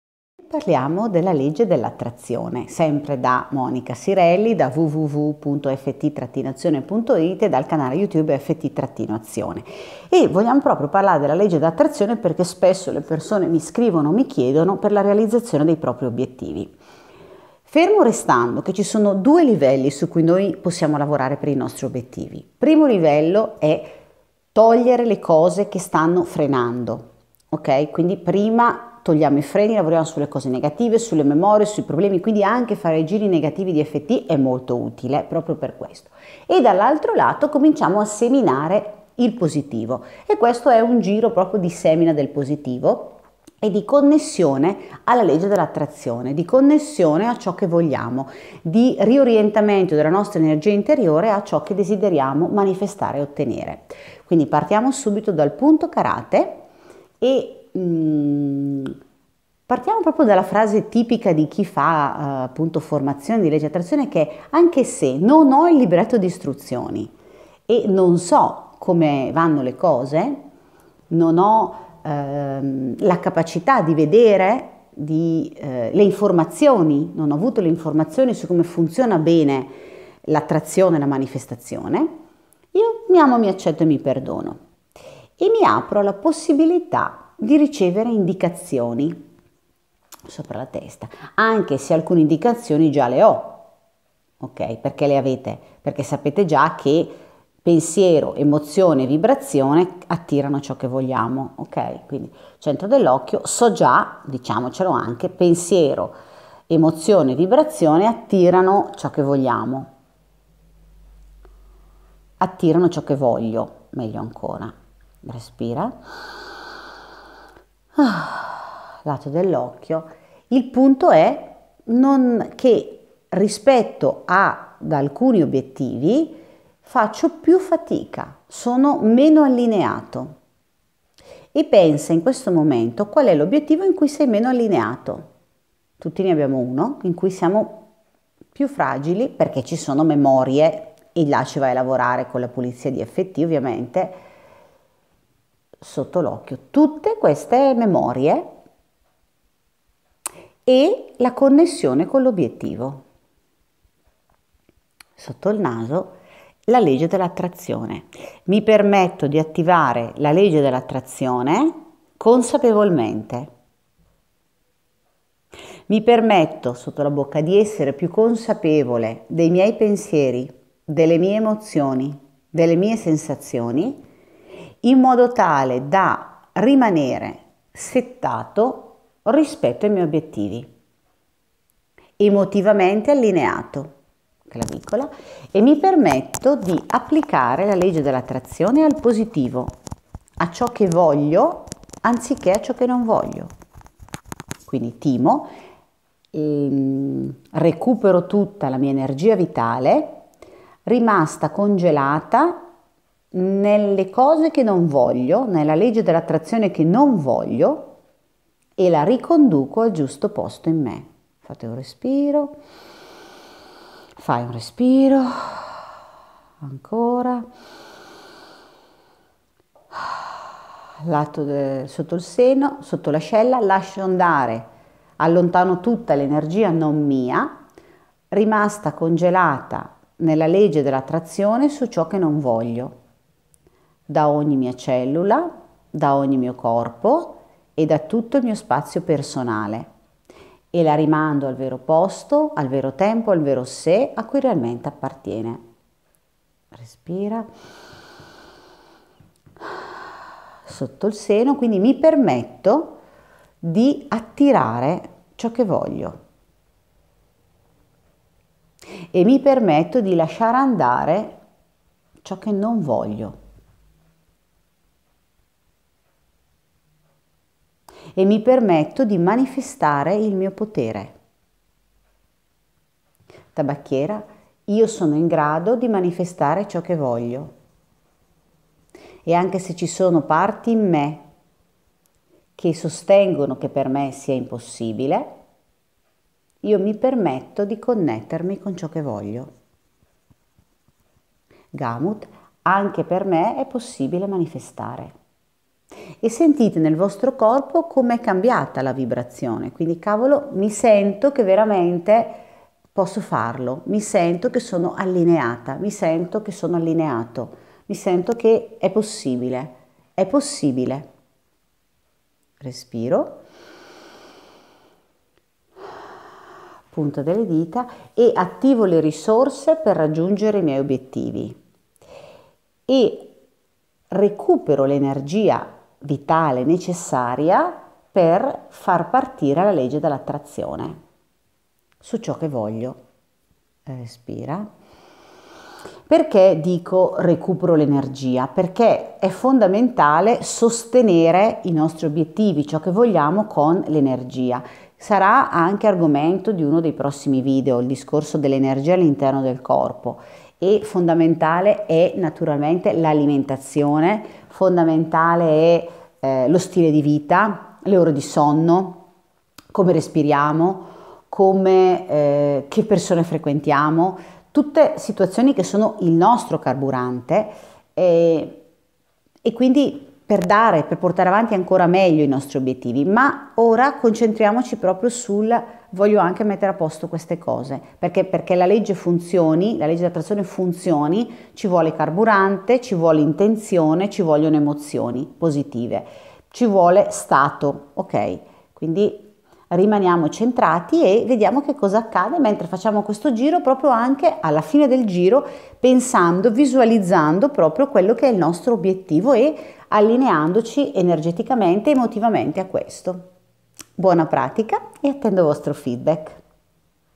Legge dell’attrazione: come manifestare con EFT
Entrare in vibrazione con ciò che vuoi è la base di ogni obiettivo: connettiti con i tuoi desideri con la pratica del video.

Attraverso la pratica di EFT proposta in questo video puoi metterti in connessione emotiva con la realizzazione per canalizzare energia e azione dove vuoi.

Buona pratica!
Monica Sirelli